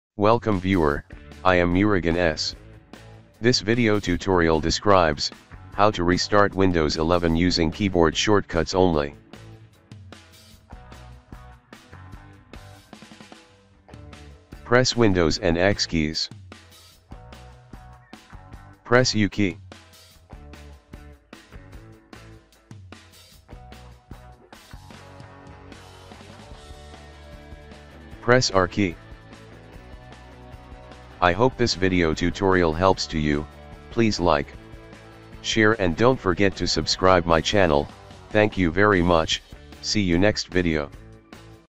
How to Restart Windows 11 Using Keyboard Shortcuts Only
This Tutorial Helps to How to Restart Windows 11 Using Keyboard Shortcuts Only

00:00 Intro
00:14 Start Menu
00:18 Shutdown or Sign Out
00:27 Restart
00:30 Closing